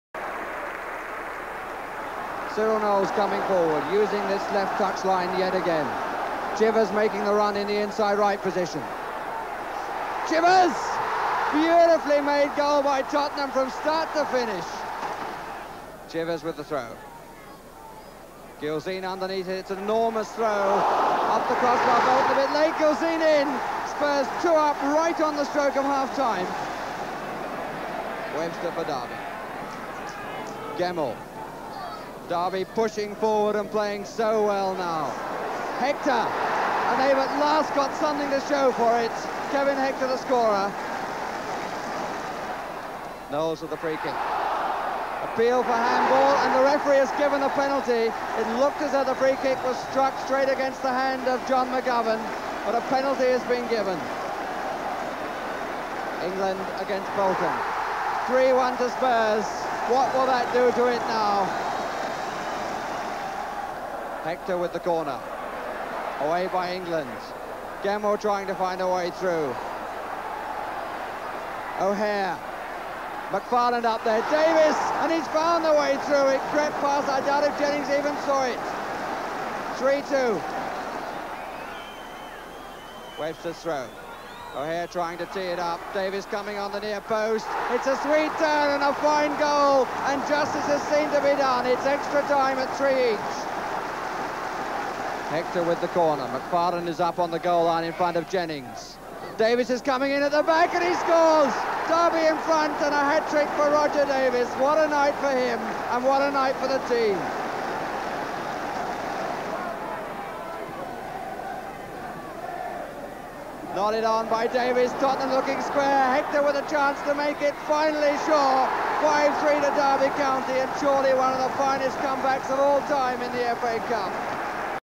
1972-73 - Tottenham Hotspur 3 Derby County 5 - 07/02/1973 - FA Cup 4th Round Replay
After extra time
Derby scorers - Kevin Hector 2 , Roger Davies 3
Spurs scorers - Martin Chivers , Alan Gilzean , Mike England (penalty)
Attendance 52,736